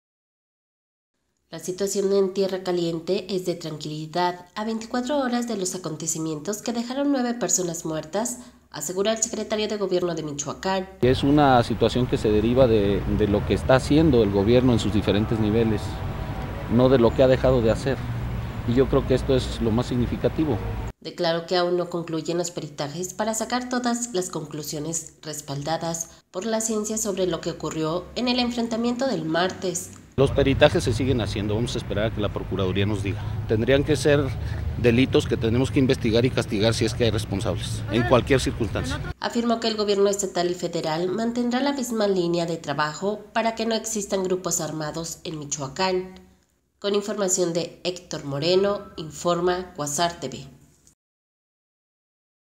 En calma Tierra Caliente de Michoacán asegura gobierno
La situación en Tierra Caliente es de tranquilidad a 24 horas de los acontecimientos que dejaron nueve personas muertas, asegura el secretario de gobierno de Michoacán...